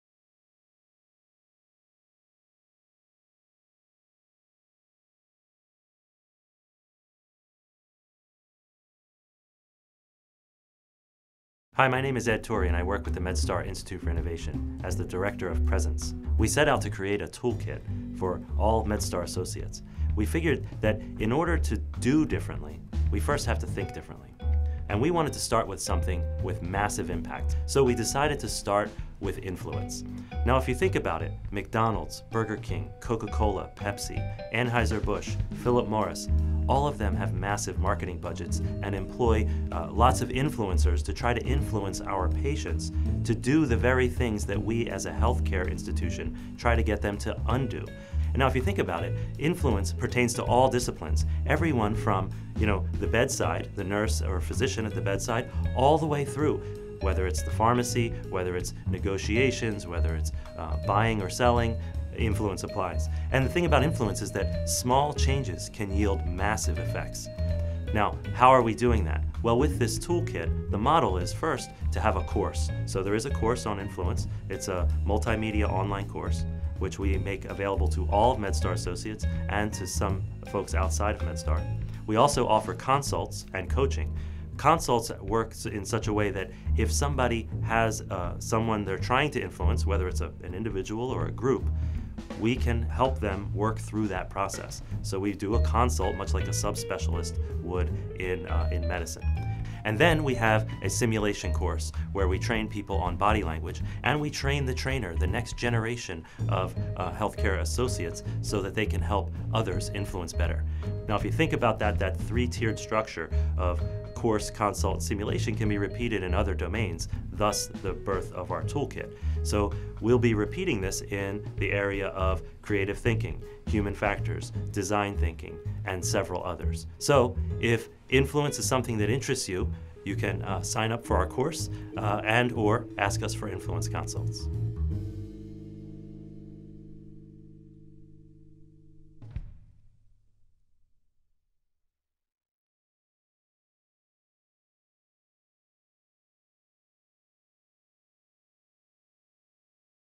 Influence with Ed Tori
Taste of Innovation - Influence with Ed Tori

Ed Tori, D.O., FACP, CH is the Director of Presence, MedStar Institute for Innovation.